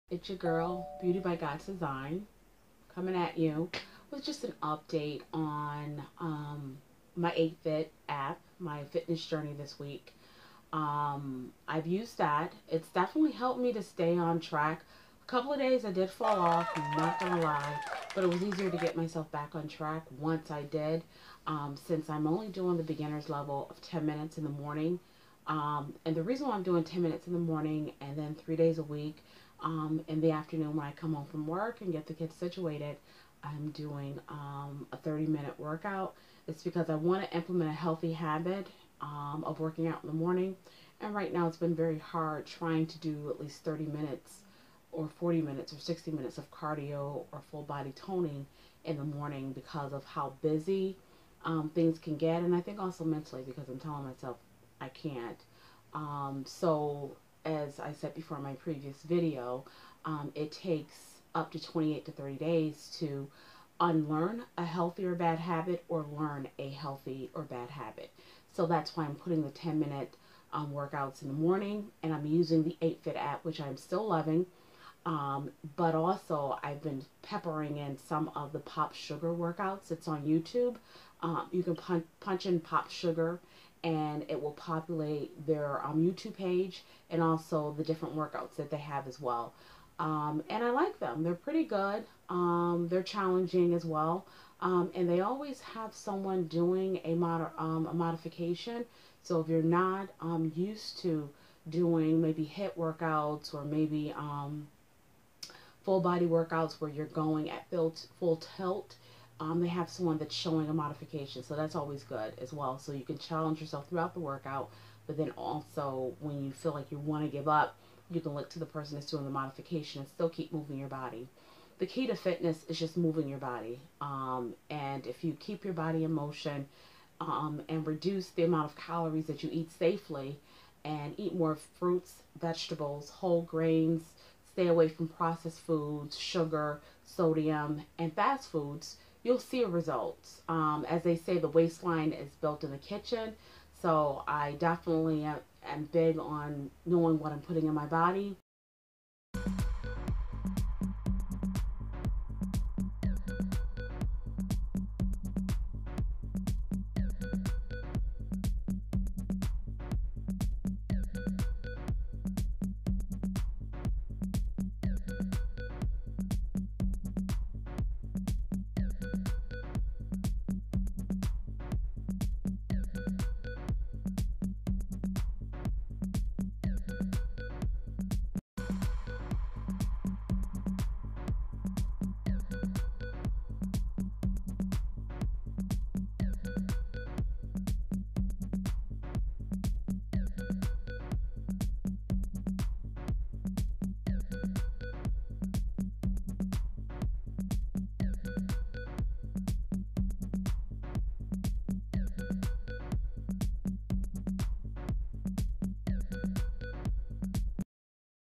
How to stay motivated Week 1 Fitness Journey!
Reflections on my completed week 1 of my fitness journey. I share tips to how I stay motivated and my fitness routine.

Video created by using Cyber Link Power Director. All music and sound-effects were provided by Power Director.